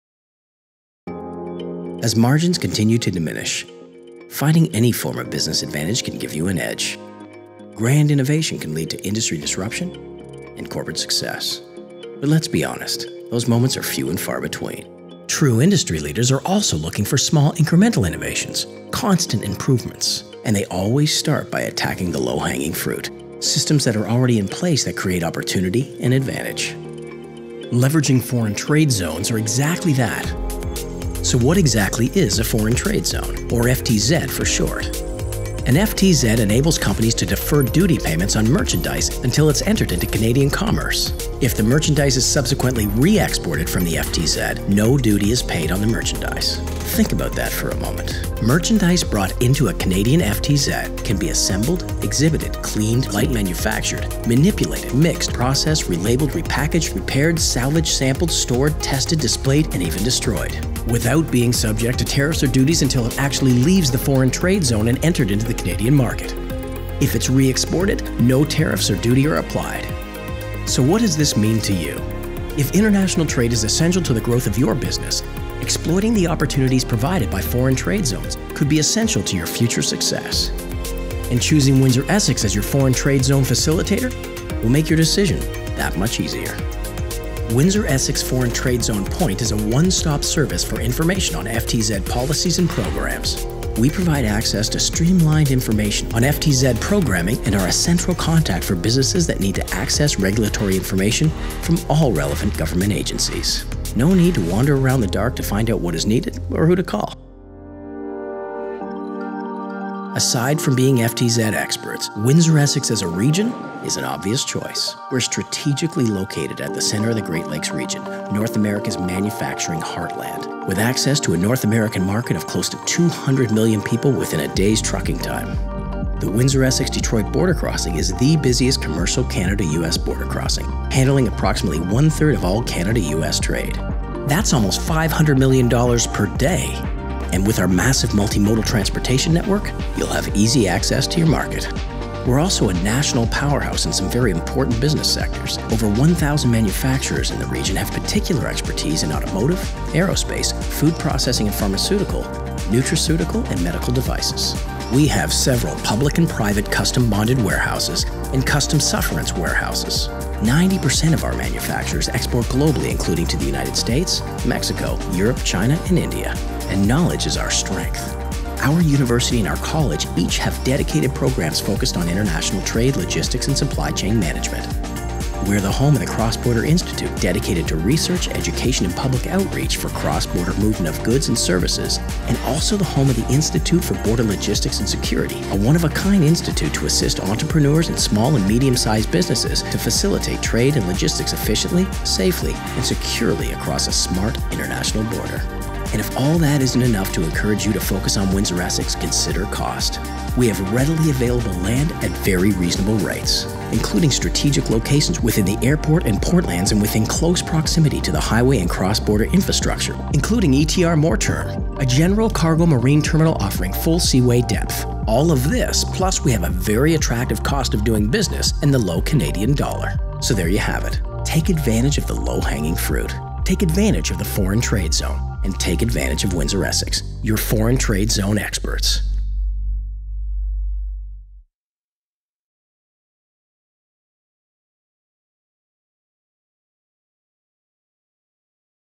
Windsor-Essex Foreign Trade Zone
Windsor-Essex is designated as a Foreign Trade Zone (FTZ), which allows tax and tariff exemptions to promote cross-border business and production. Invest WindsorEssex leads the Foreign Trade Zone programs for the region and is a one-stop-shop for information on Canada's FTZ policies and programs.